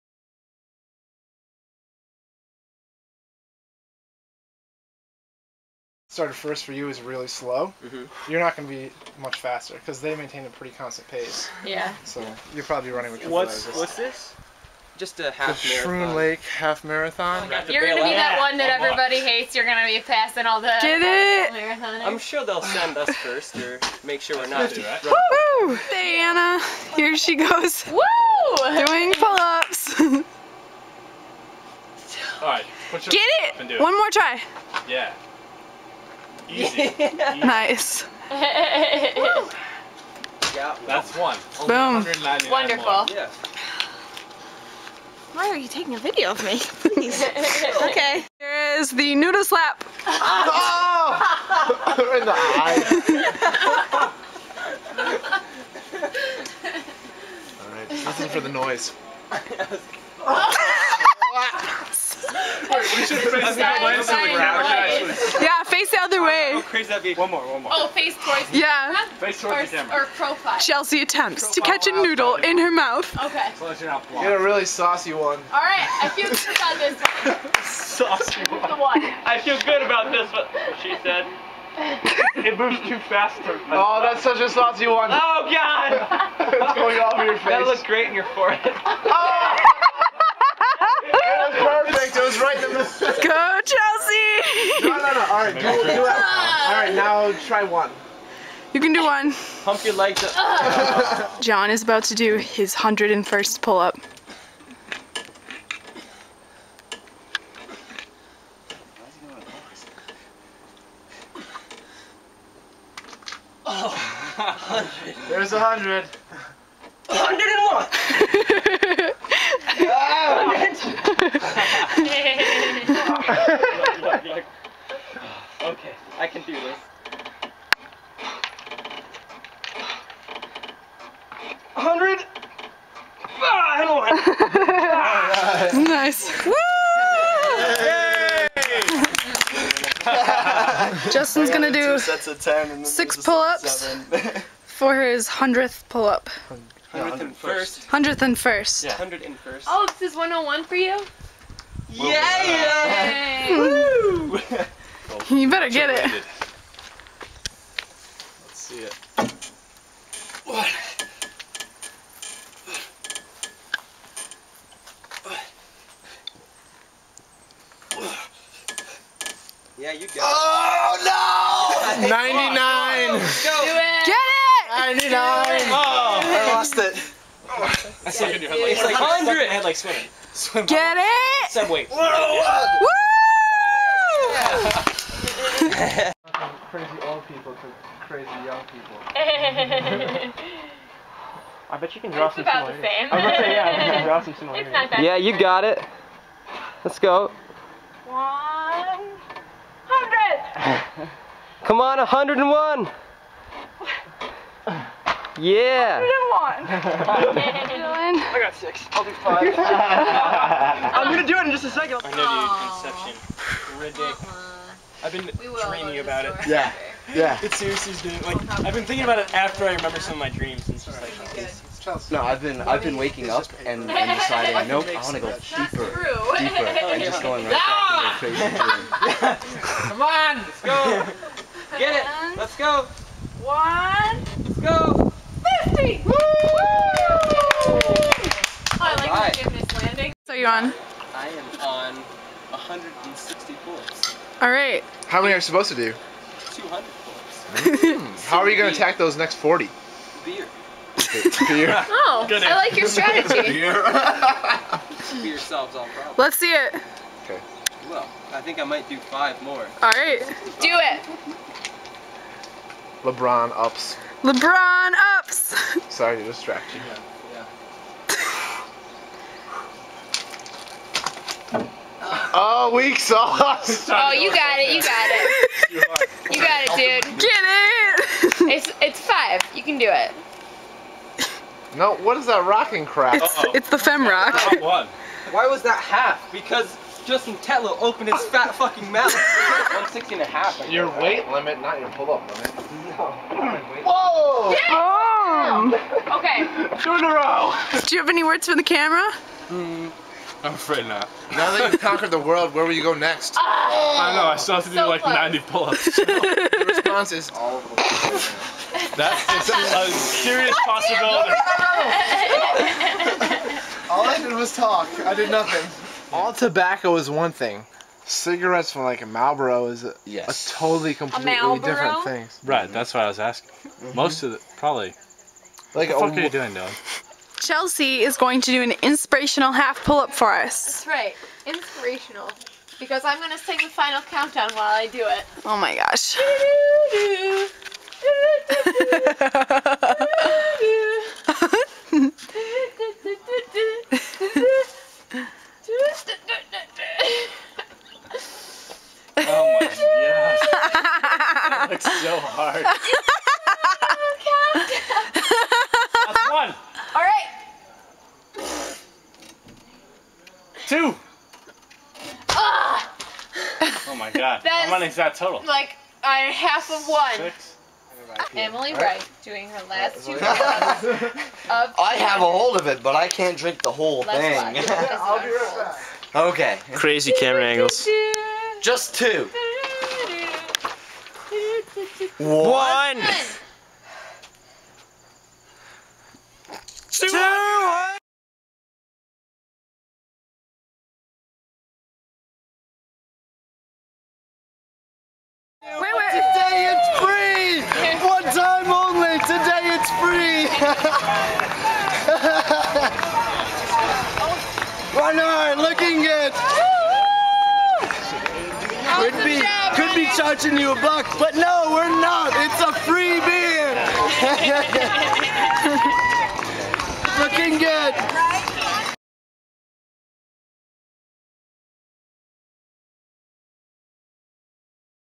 200 pull-up challenge
We attempted to do 200 pull-ups as quickly as possible during the Ironman weekend in Lake Placid while drinking and enjoying the Ironman Triathalon.